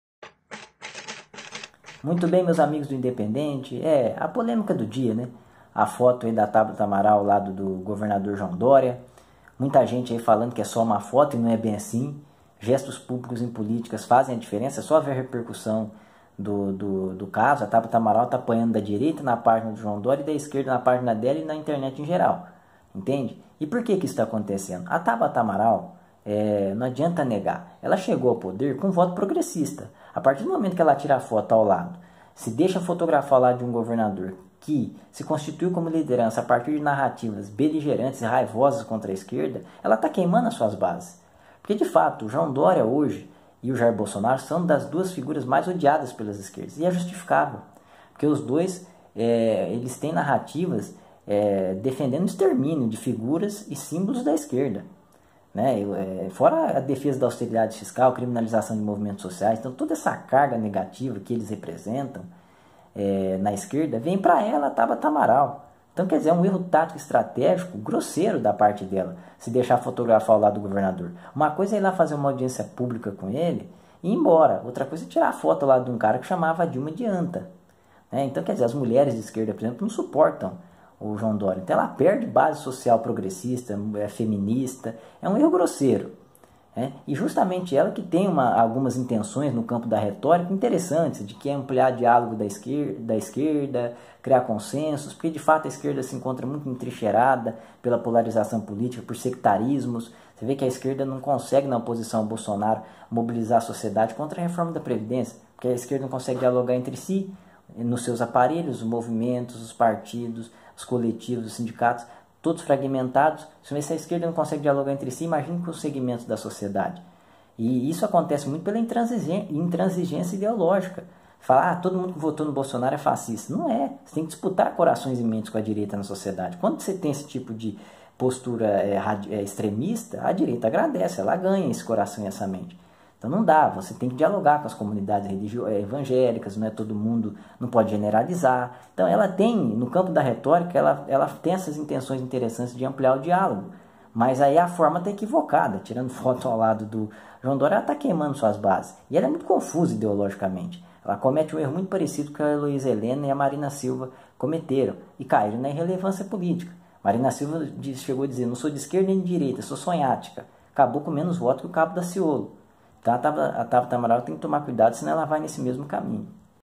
Tabata Amaral e João Dória
A deputada federal Tabata Amaral (PDT-SP) se encontrou com o governador de SP, João Doria (PSDB), na segunda-feira (22).

Tabata afirmou que eles conversaram sobre o “combate à violência contra a mulher e educação, especialmente técnica e profissional”, enquanto Doria destacou um diálogo sobre a “importância da aprovação” da Reforma da Previdência.

O jornalista e cientistas político, André Henrique, analisou o encontro e seus impactos políticos.

| Ajude o Independente a produzir conteúdos para informação e conscientização das pessoas